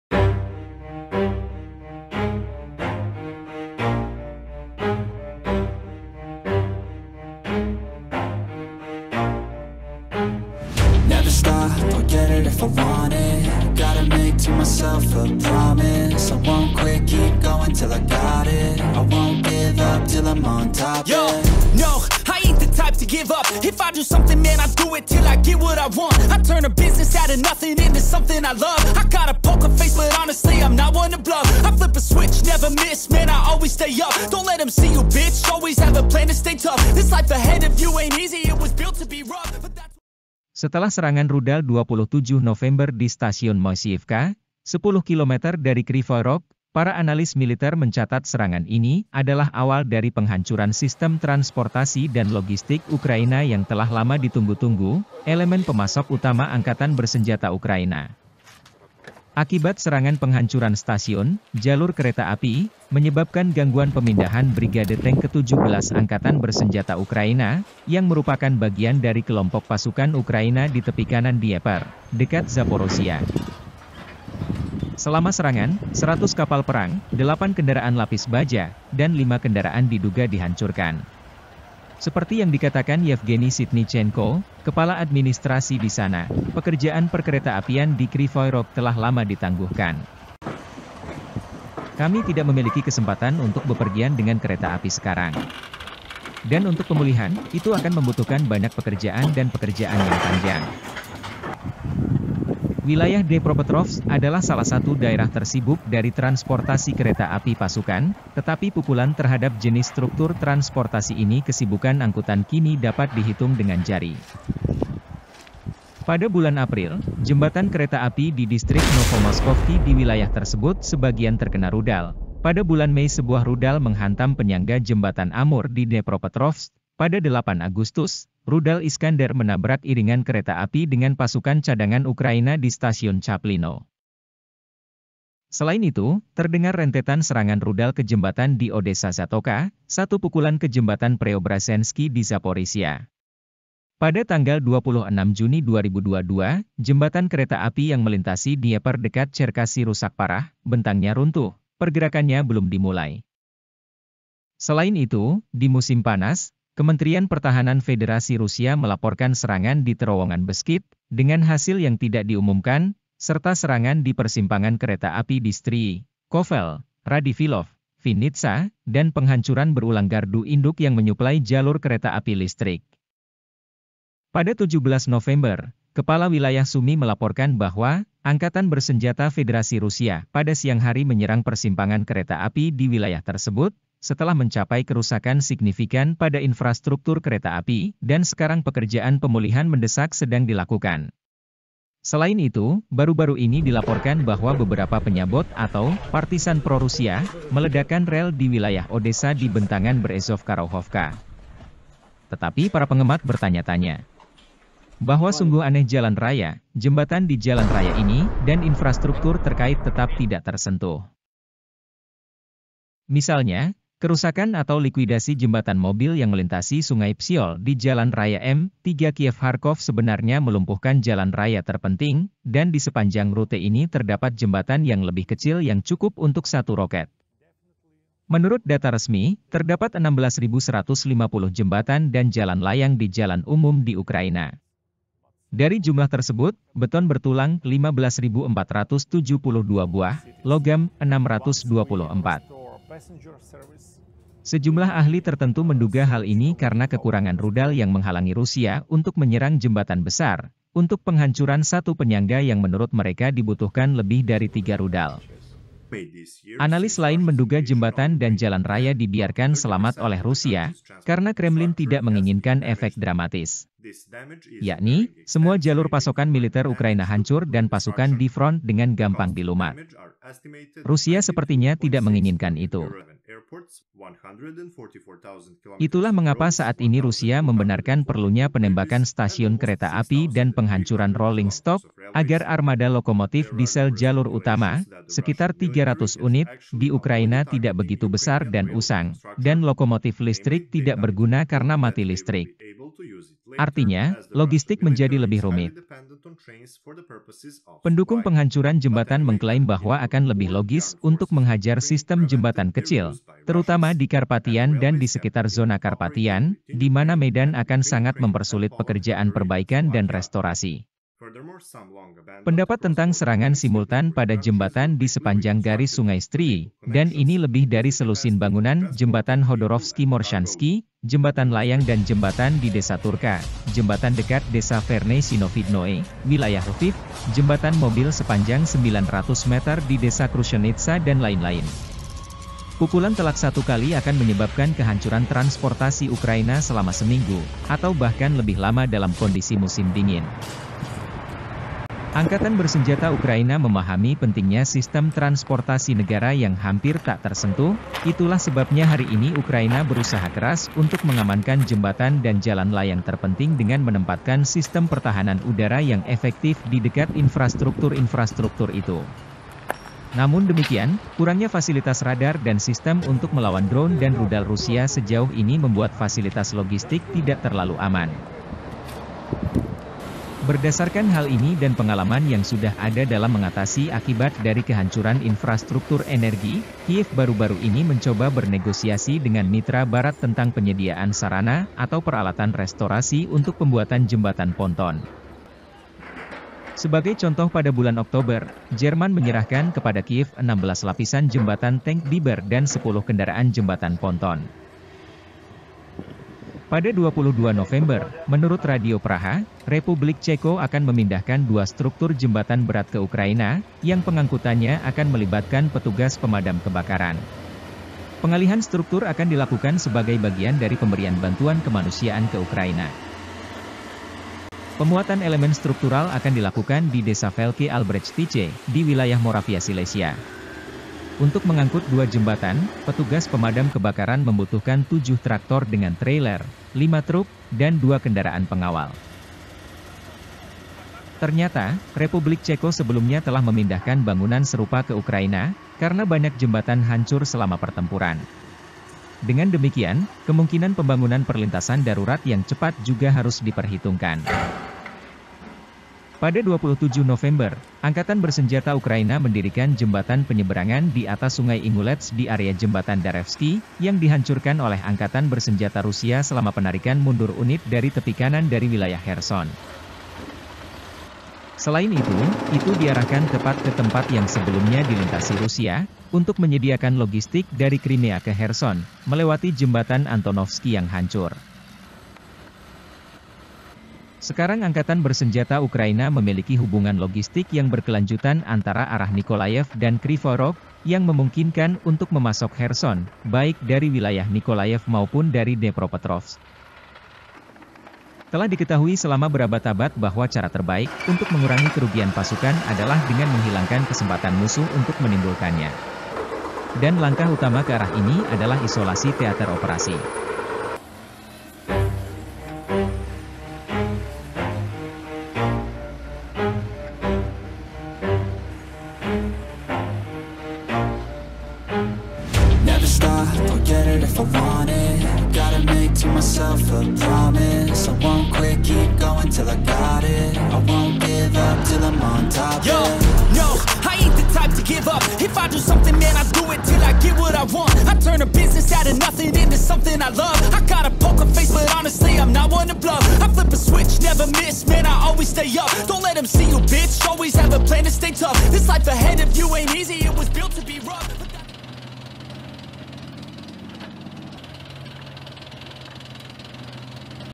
Pertempuran Jembatan dan Rel Kereta Api
Setelah serangan rudal 27 November di stasiun Moisiivka, sepuluh kilometer dari Krivoy Rog, para analis mencatat serangan adalah awal dari penghancuran sistem transportasi dan logistik Ukraina yang telah lama ditunggu-tunggu, elemen pemasok utama Angkatan Bersenjata Ukraina.
Akibat serangan penghancuran stasiun, jalur kereta api, menyebabkan gangguan pemindahan brigade tank ke-17 Angkatan Bersenjata Ukraina, yang merupakan bagian dari kelompok pasukan Ukraina di tepi kanan Dnieper, dekat Zaporoziya.
Selama serangan, 100 kapal perang, 8 kendaraan lapis baja, dan 5 kendaraan diduga dihancurkan. Seperti yang dikatakan Yevgeny Sitnichenko, kepala administrasi di sana, pekerjaan perkeretaapian di Krivoy Rog telah lama ditangguhkan:
“Kami tidak memiliki kesempatan untuk bepergian dengan kereta api sekarang. Dan untuk pemulihan, itu akan membutuhkan banyak pekerjaan dan pekerjaan yang panjang.”
Wilayah Dnepropetrovsk adalah salah satu daerah tersibuk dari transportasi kereta api pasukan, tetapi pukulan terhadap jenis struktur transportasi ini kesibukan angkutan kini dapat dihitung dengan jari.
Pada bulan April, jembatan kereta api di distrik Novomoskovsky di wilayah tersebut sebagian terkena rudal, pada bulan Mei sebuah rudal menghantam penyangga jembatan Amur di Dnepropetrovsk, pada 8 Agustus, rudal Iskander menabrak iringan kereta api dengan pasukan cadangan Ukraina di stasiun Chaplino.
Selain itu, terdengar rentetan serangan rudal ke jembatan di Odessa Zatoka, satu pukulan ke Jembatan Preobrazhensky di Zaporizhzhya. Pada tanggal 26 Juni 2022, jembatan kereta api yang melintasi Dnieper dekat Cherkassy rusak parah (bentangnya runtuh), pergerakannya belum dimulai.
Selain itu, di musim panas, Kementerian Pertahanan Federasi Rusia melaporkan serangan di terowongan Beskid, dengan hasil yang tidak diumumkan, serta serangan di persimpangan kereta api di Stryi, Kovel, Radivilov, Vinnitsa, dan penghancuran berulang gardu induk yang menyuplai jalur kereta api listrik.
Pada 17 November, kepala wilayah Sumy melaporkan bahwa "Angkatan Bersenjata Federasi Rusia pada siang hari menyerang persimpangan kereta api di wilayah tersebut, setelah mencapai kerusakan signifikan pada infrastruktur kereta api, dan sekarang pekerjaan pemulihan mendesak sedang dilakukan."
Selain itu, baru-baru ini dilaporkan bahwa beberapa penyabot atau "partisan pro-Rusia" meledakkan rel di wilayah Odessa di bentangan Berezovka-Raukhovka.
Sungguh aneh bahwa jalan raya, jembatan di jalan raya ini, dan infrastruktur terkait tetap tidak tersentuh.
Misalnya, kerusakan atau likuidasi jembatan mobil yang melintasi Sungai Psyol di jalan raya M-3 Kiev-Kharkov sebenarnya melumpuhkan jalan raya terpenting, dan di sepanjang rute ini terdapat jembatan yang lebih kecil yang cukup untuk satu roket.
Menurut data resmi, terdapat 16.150 jembatan dan jalan layang di jalan umum di Ukraina. Dari jumlah tersebut, beton bertulang - 15472 buah, logam - 624.
Sejumlah ahli tertentu menduga hal ini  karena kekurangan rudal yang menghalangi Rusia untuk menyerang jembatan besar, untuk penghancuran satu penyangga yang menurut mereka dibutuhkan lebih dari tiga rudal.
Analis lain menduga jembatan dan jalan raya dibiarkan selamat oleh Rusia, karena Kremlin tidak menginginkan efek dramatis. Yakni, semua jalur pasokan militer Ukraina hancur dan pasukan di front dengan gampang dilumat. Rusia sepertinya tidak menginginkan itu.
Itulah mengapa saat ini Rusia membenarkan perlunya penembakan stasiun kereta api dan penghancuran rolling stock, Agar armada lokomotif diesel jalur utama (hingga 300 unit) di Ukraina tidak begitu besar dan usang, dan lokomotif listrik tidak berguna karena mati listrik. Artinya, logistik menjadi lebih rumit.